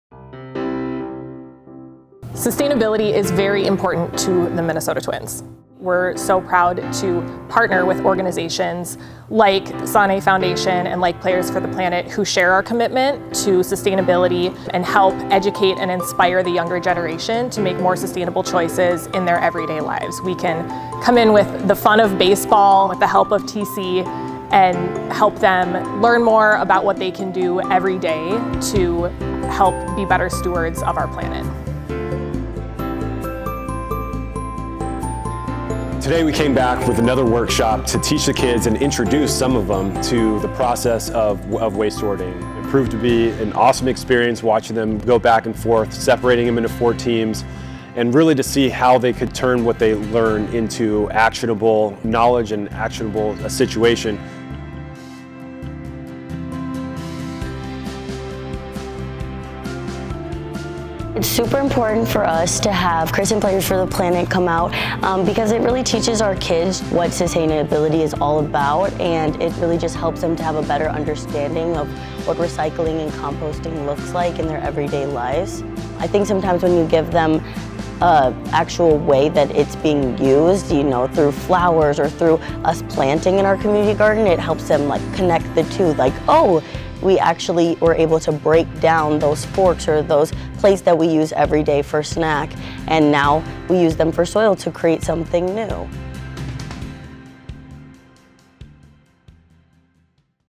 Twins partner with Players for the Planet
Our commitment to sustainability extends beyond Target Field and throughout our community.

We partnered with Players For The Planet and The Sanneh Foundation to teach kids how they can be champions for our environment.